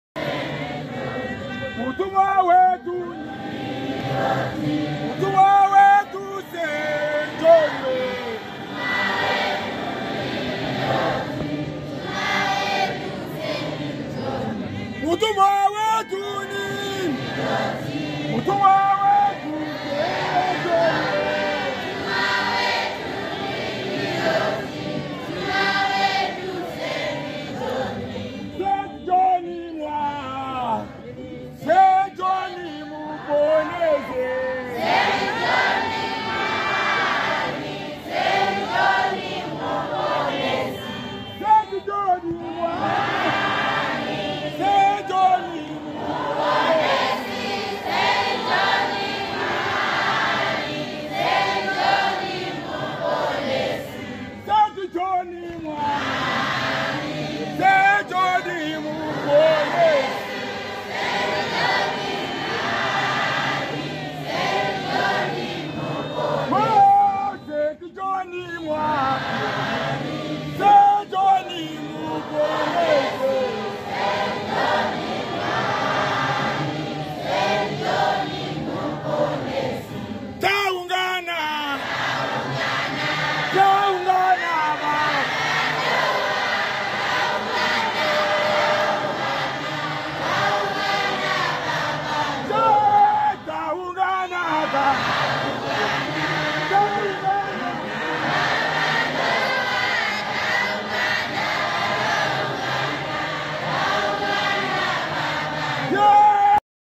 Passover service at Johane Marange Apostolic Church
Johane Marange Apostolic Church service during Passover at Mafararikwa Shrine in Marange

📹 Moffat Mungazi